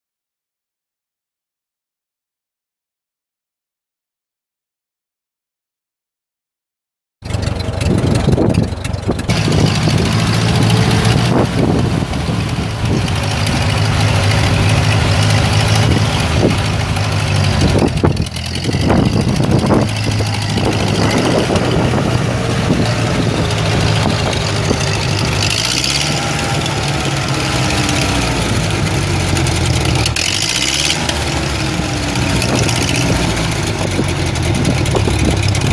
1948 Ford 8N tractor for sale | no-reserve Internet auction December 28, 2016
View a detailed listing of this item and place bids

1948 Ford 8N tractor
Ford four cylinder gas engine
Manual transmission
4F-1R gears
Three point
Top link
540 PTO
12.4-28 rear tires
It's located in Bartlesville, OK, and we're selling it to the highest bidder regardless of price.